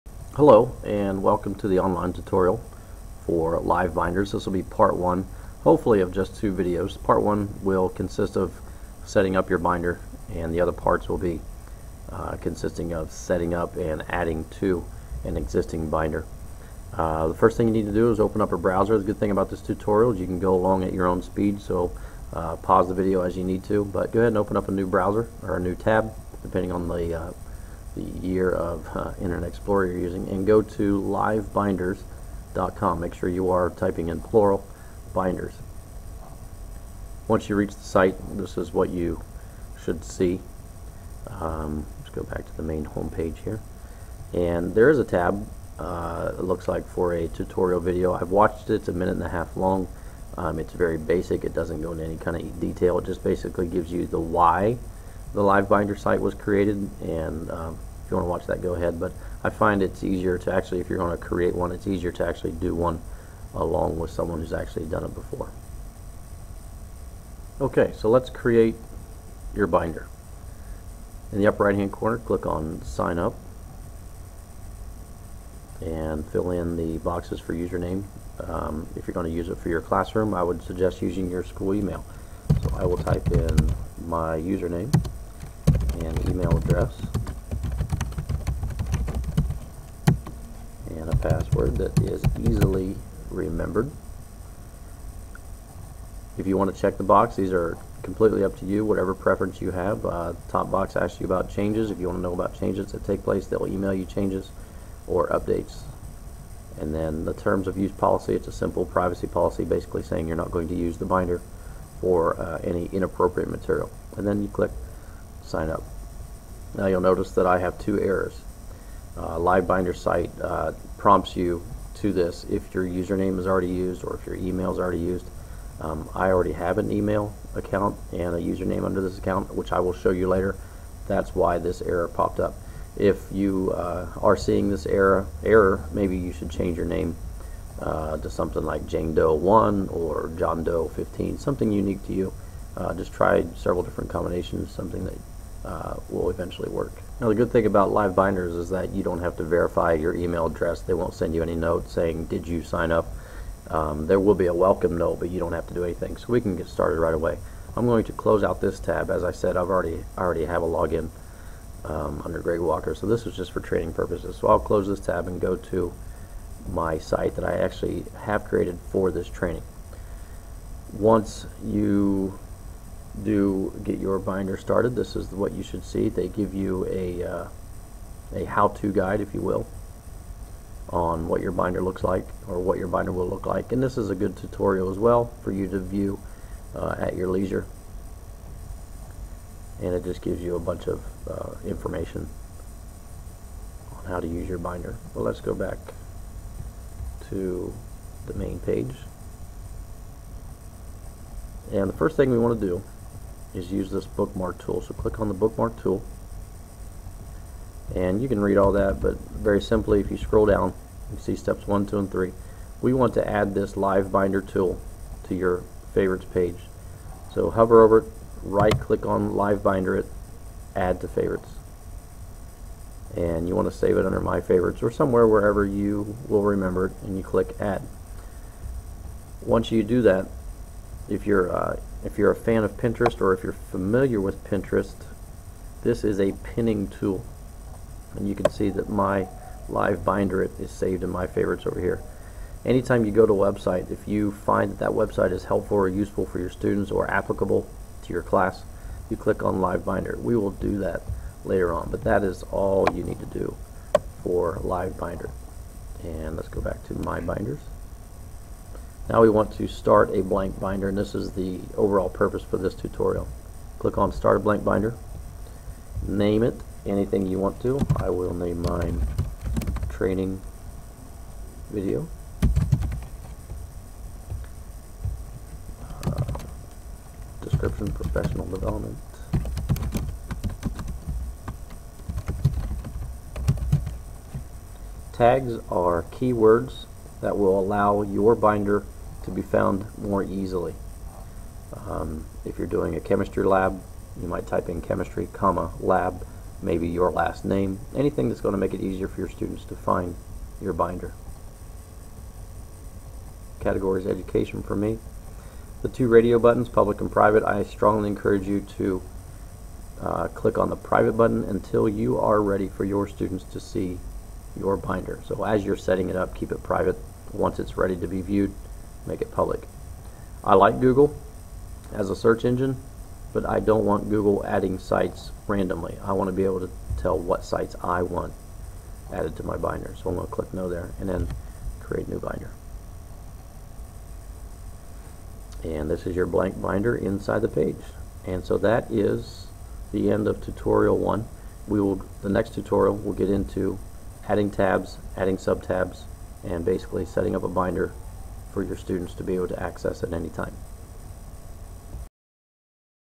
LiveBinders Tutorial 1: The Set Up
Professional Development training for Nixa Public Schools for set up and use of LiveBinders.